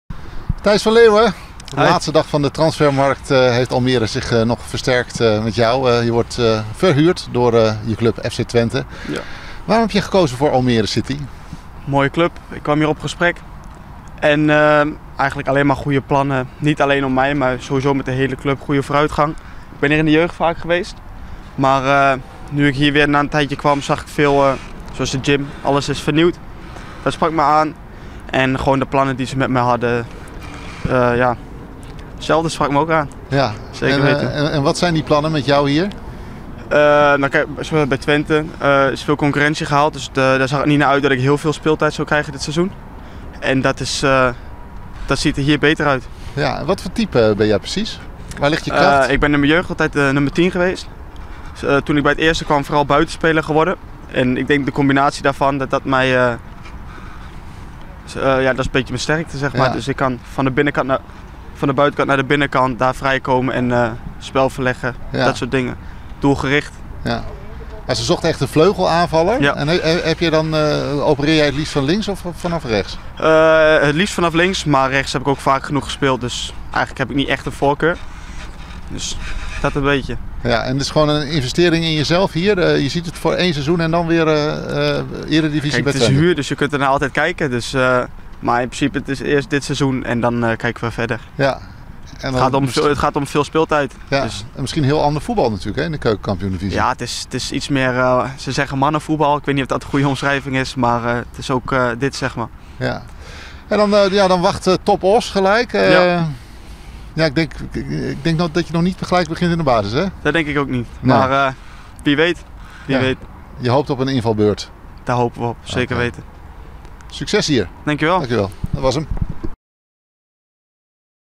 Vleugelaanvaller Thijs van Leeuwen zit bij wedstrijdselectie, maar blijft op bank
Almere City FC wil tegen TOP Oss, de verrassende nummer twee van de ranglijst, de eerste thuiszege van het nieuwe seizoen boeken in de Keuken Kampioen Divisie.

De nieuwe spelers: vleugelaanvaller Thijs van Leeuwen, afkomstig van eredivisieclub FC Twente, en linksback Thomas Poll ,afkomstig van FC Groningen, zitten bij de wedstrijdselectie, maar beginnen nog wel op de bank. De thuiswedstrijd tegen TOP Oss begint zaterdagmiddag om 16.30 uur en staat onder leiding van scheidsrechter Nagtegaal.